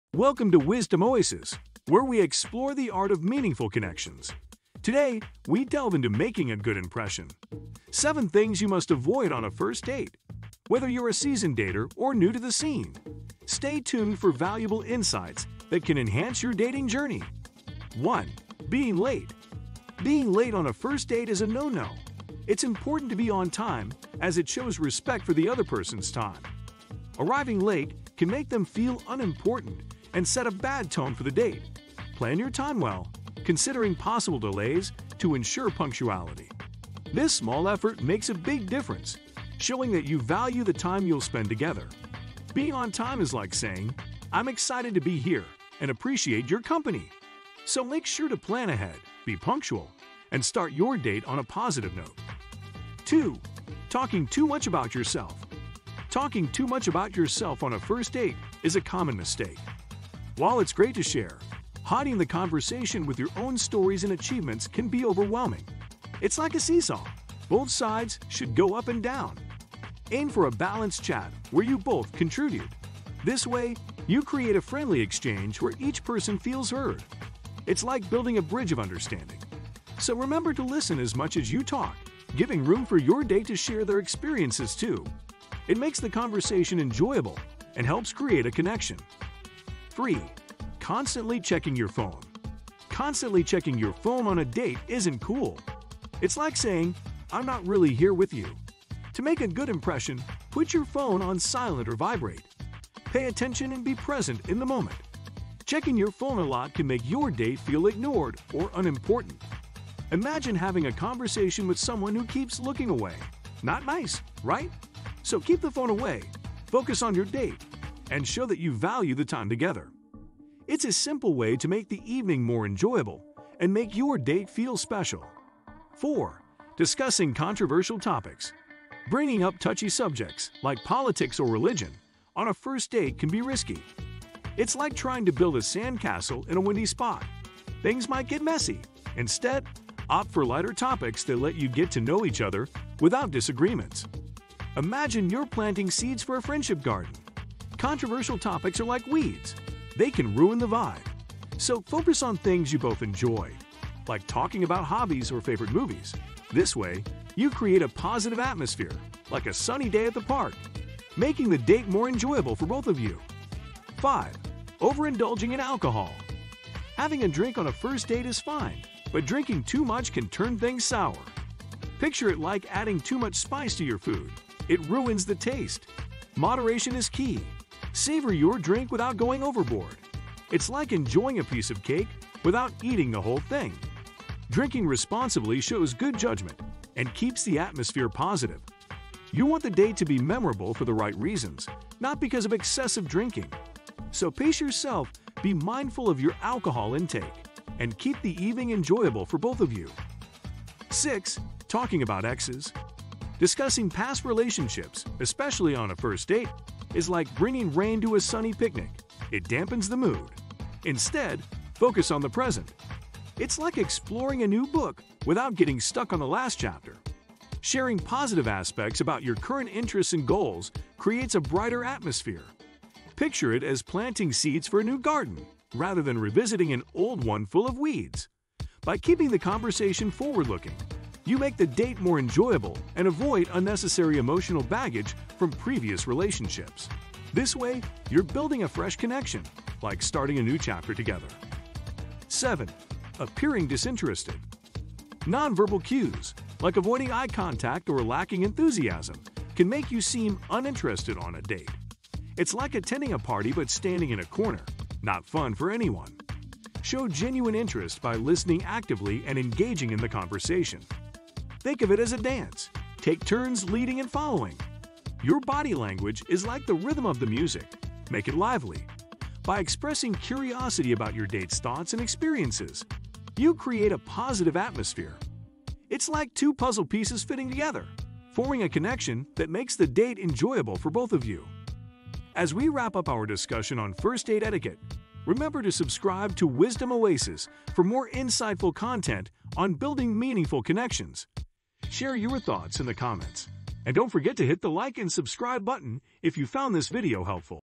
Making a Good Impression 7 Things You Must Avoid on a First Date
Embark on the journey to successful first dates with our latest video! 🌟 In "Making a Good Impression," we unveil the top 7 things you absolutely must avoid on a first date to ensure a positive and memorable experience. From awkward silences to fashion faux pas, we've got you covered with practical tips to steer clear of common pitfalls. Watch now for expert advice on creating a lasting impression and building a strong foundation for potential relationships. Don't let simple mistakes ruin your chance at love – hit play and elevate your first date game today! 💑✨